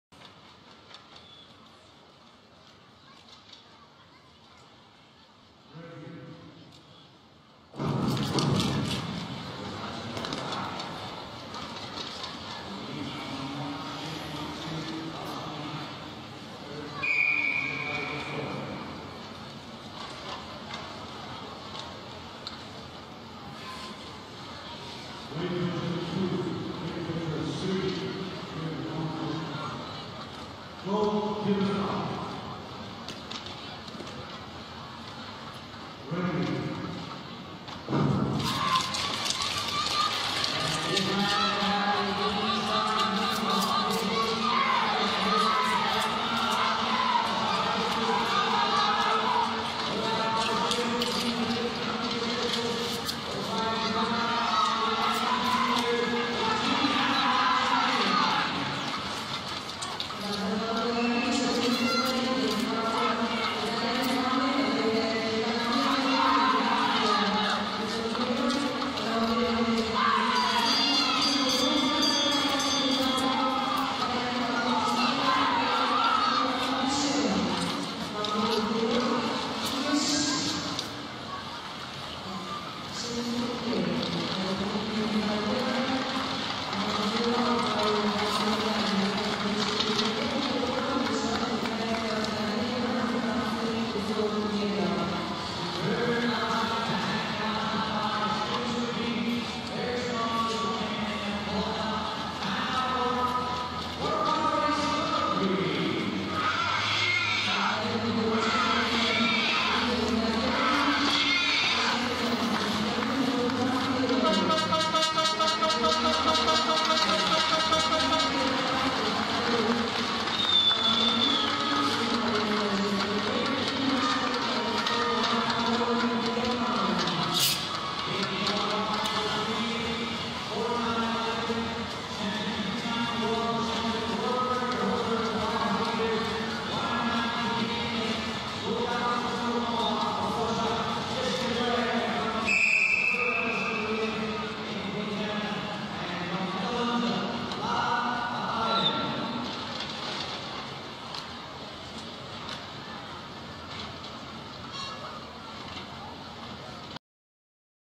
2013/2014 쇼트트랙 4차 월드컵(Kolomna) 여자 500m 준준결승 2조(심석희)
Place # Name ISU Member Results Qual.
1 63 SHIM Suk Hee KOR 43.3 Q
2 19 LIU Qiuhong CHN 43.657 Q
3 51 VALCEPINA Martina ITA 43.83
4 67 SEREIKAITE Agne LTU 44.365